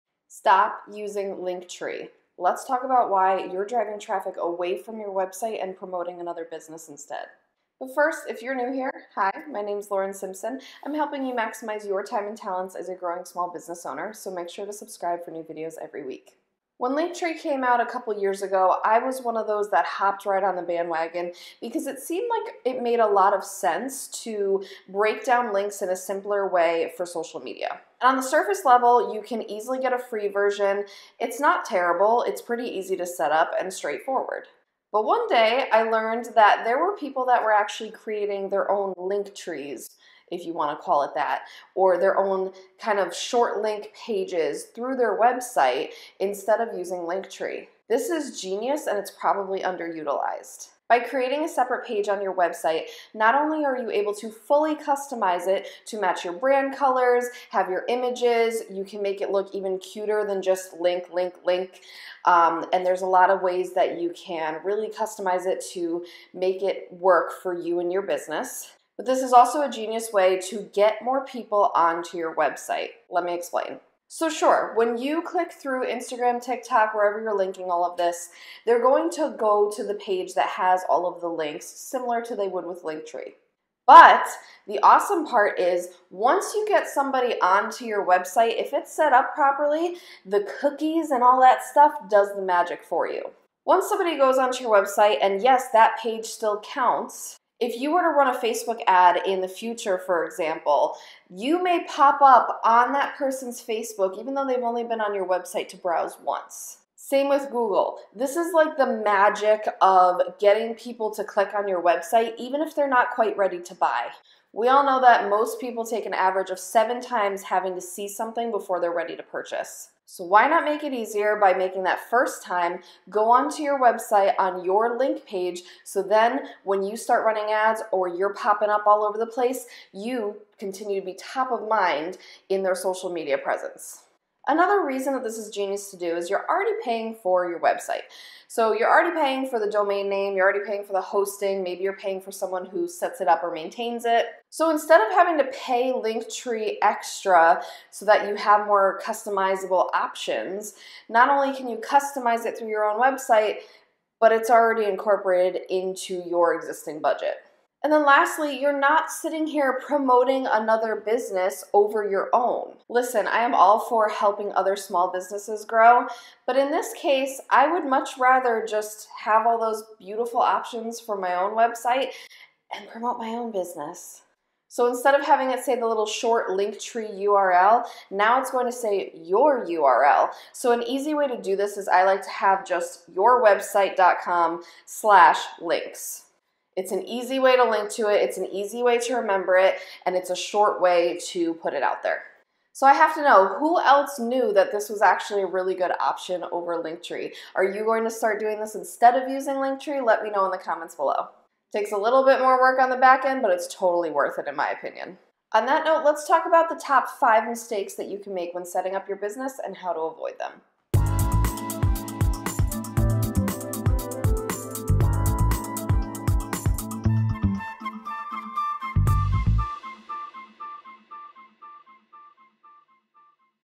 Stop Using LinkTree!
If you're still using LinkTree to share short links on social media platforms like Instagram and TikTok, you're missing out on real customers that could be benefiting from a trip to your website instead! Learn how to set up this simple page on your own website and stop missing out on profit!